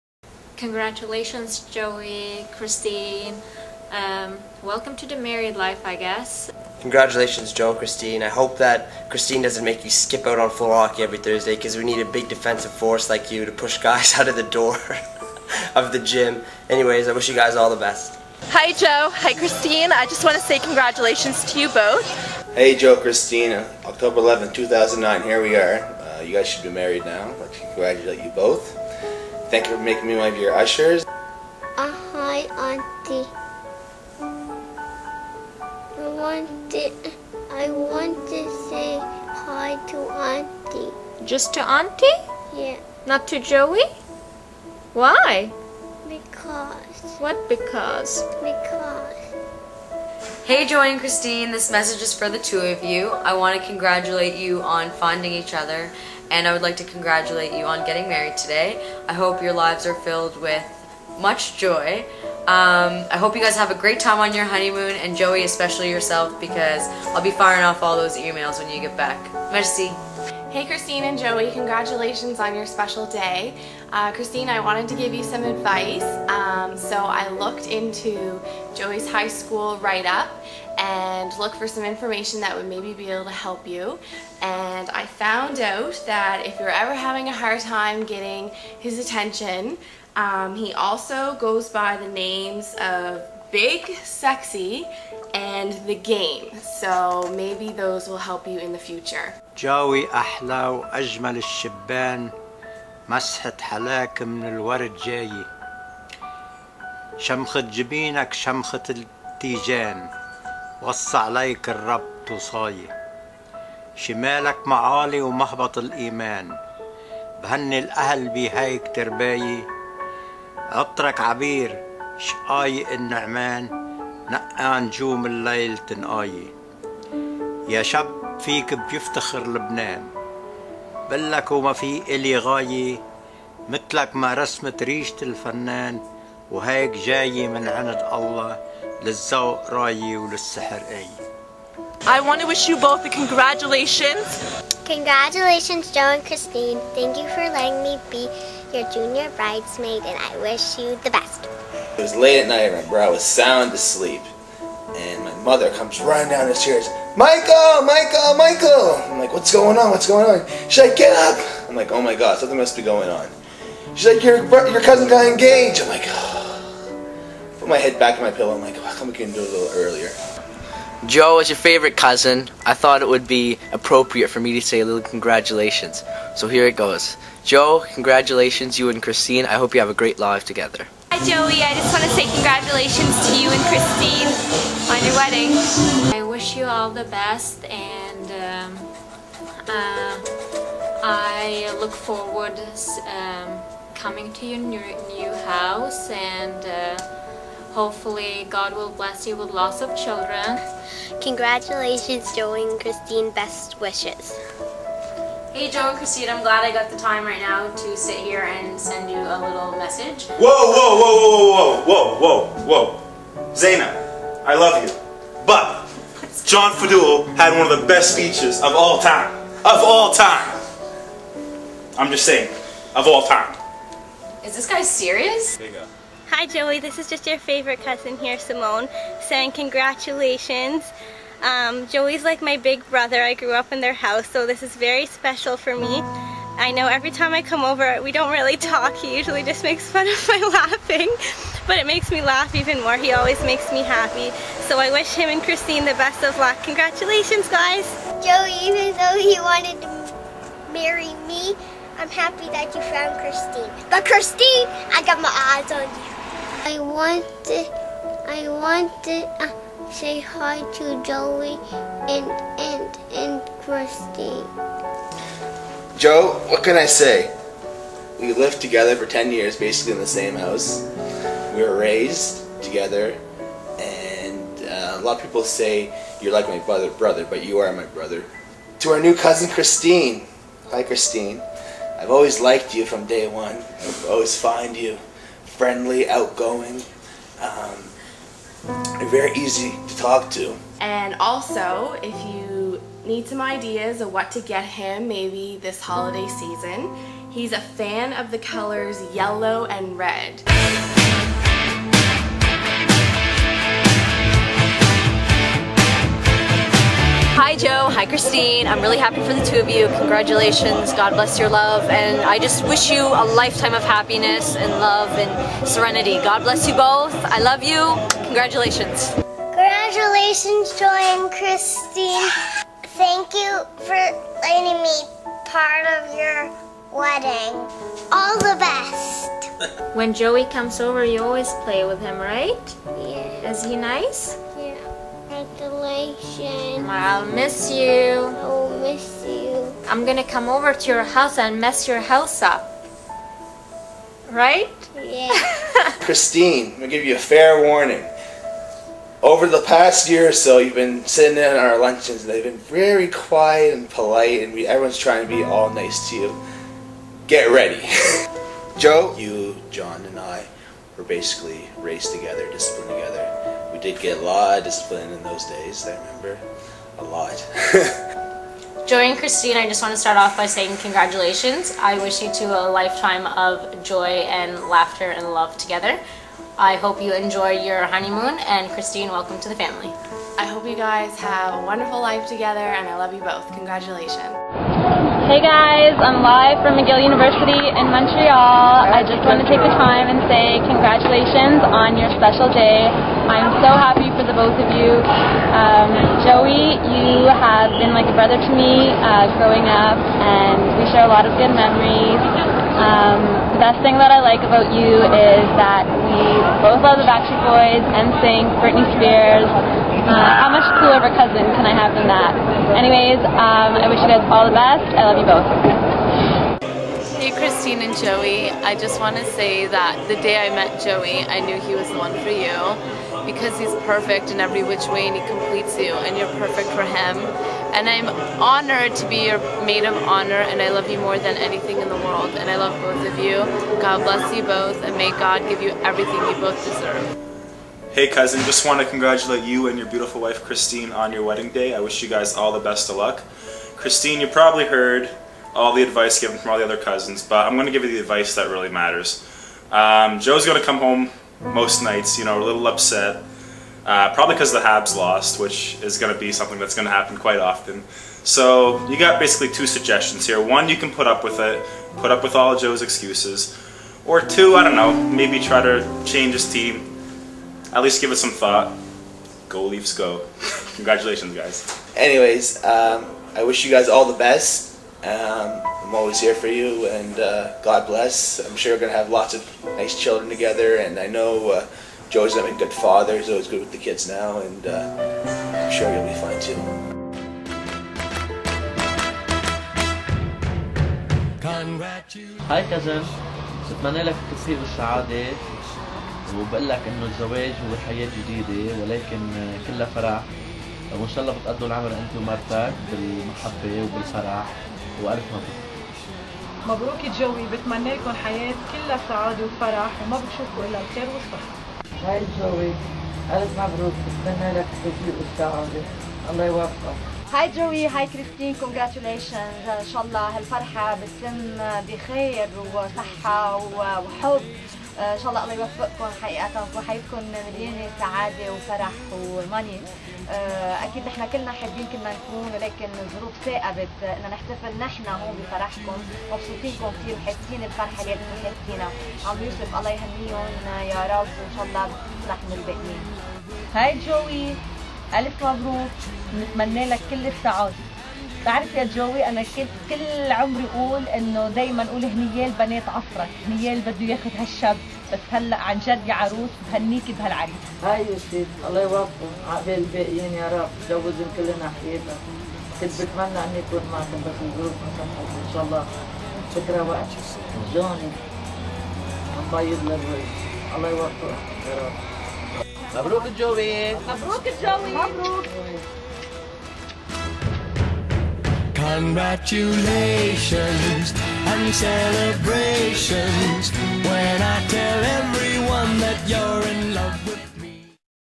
John Faddoul - Joe Wedding Shout-Outs
This is a video I prepared for my brother featuring all of his friends and cousins giving a special message to him and his new wife on their wedding day.